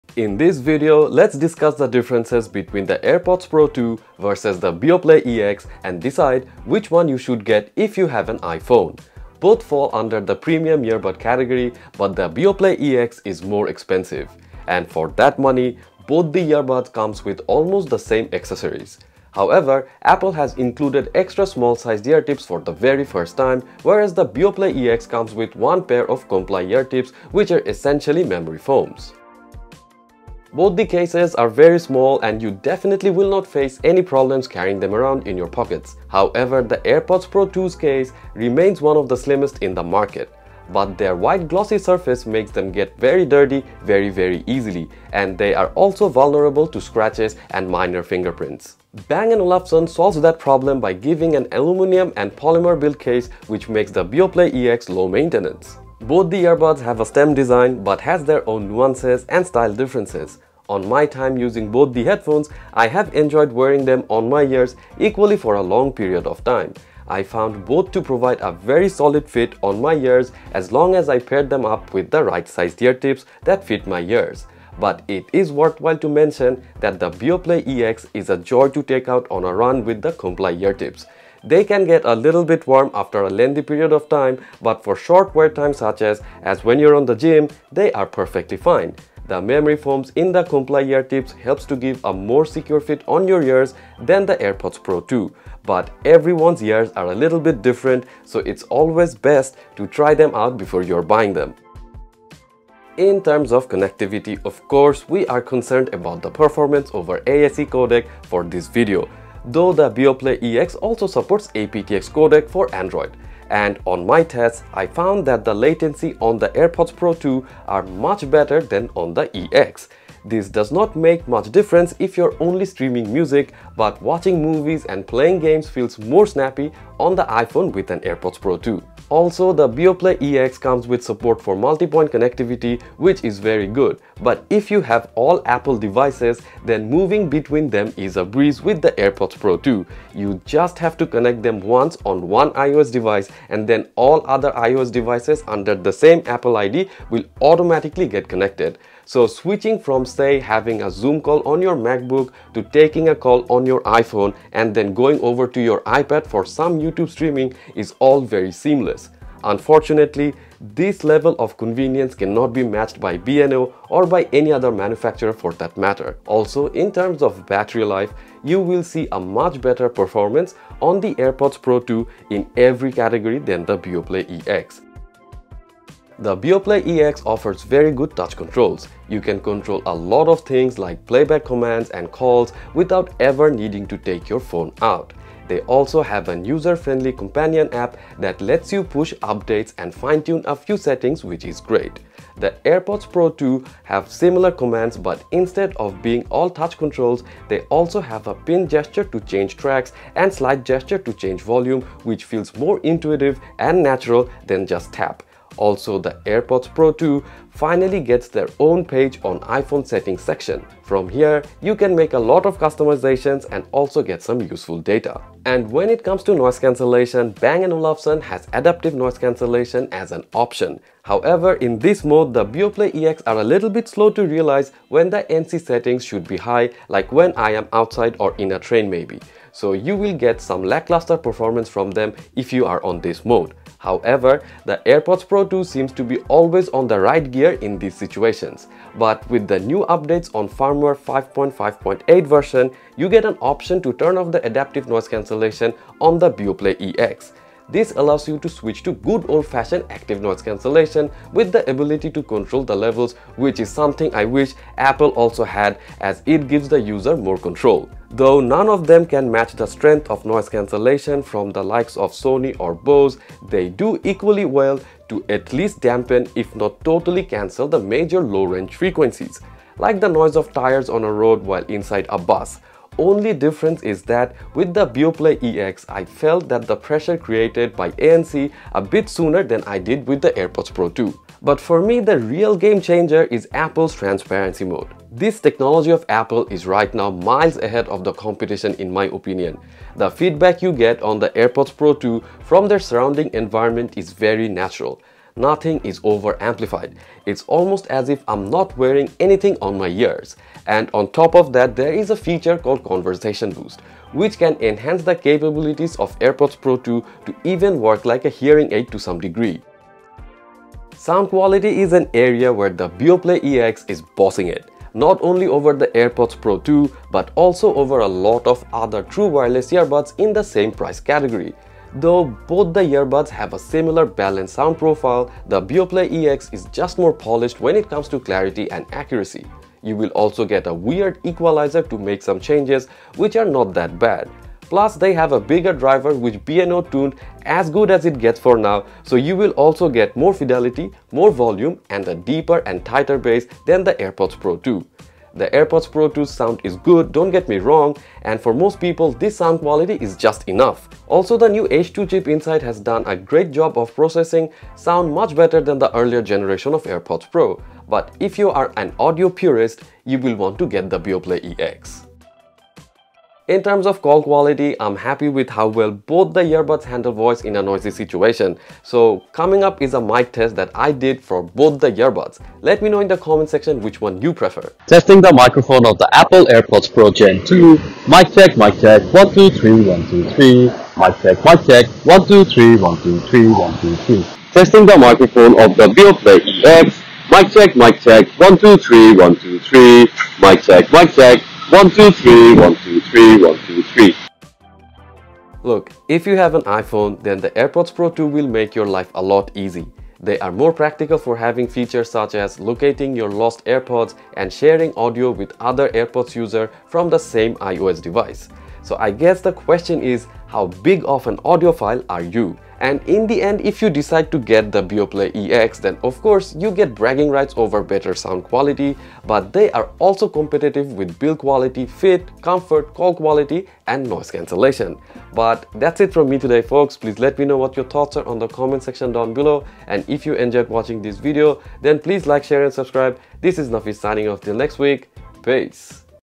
iPhone User Perspective | AirPods Pro 2 VS Beoplay EX
00:00 Intro
00:29 Portability, Design & Comfort
01:42 Battery & Connectivity
02:55 Controls
03:36 Noise Cancellation
05:10 Sound
06:02 Mic test
06:34 Conclusion